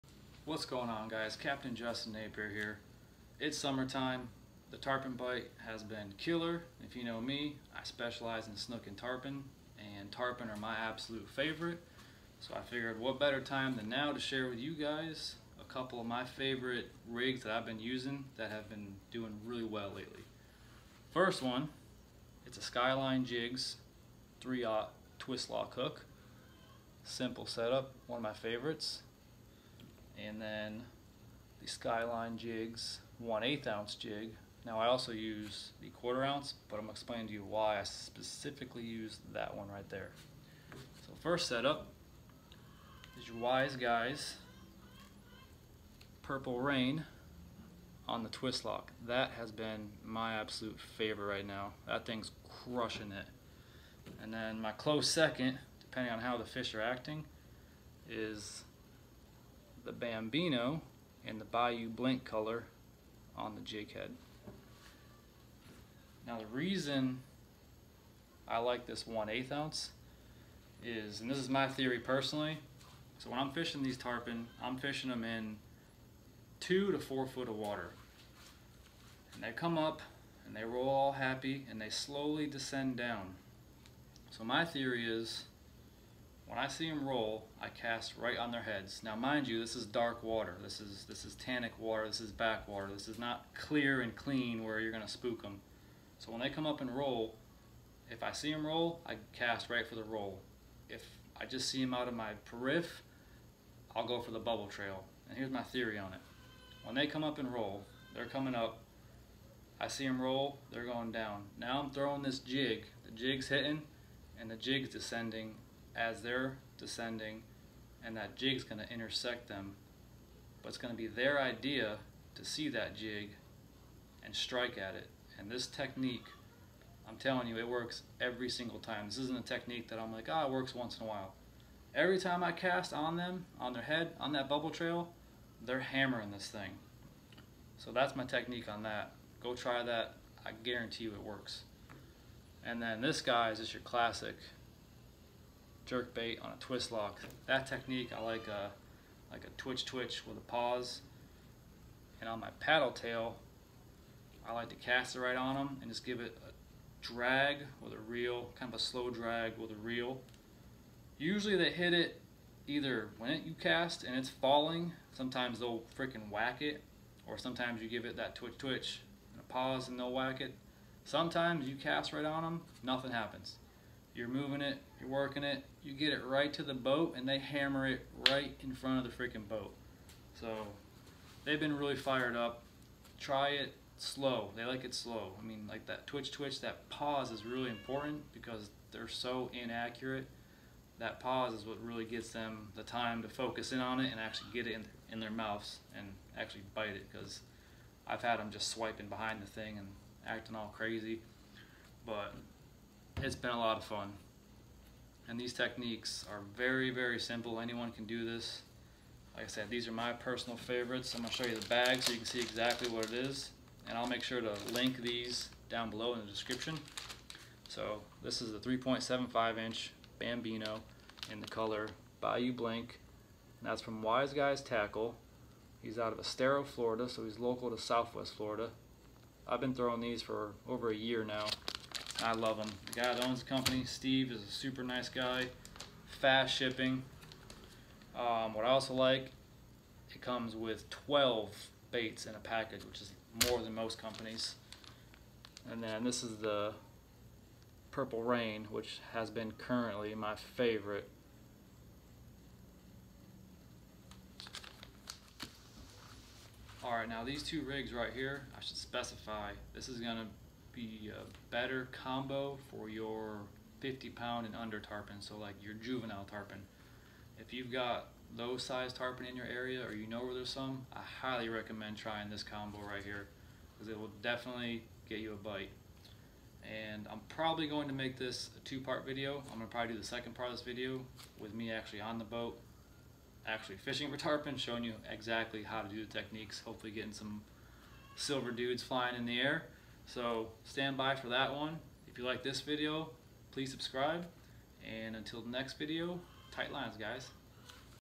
The best juvenile tarpon technique for murky water
My two favorite juvenile tarpon lure combos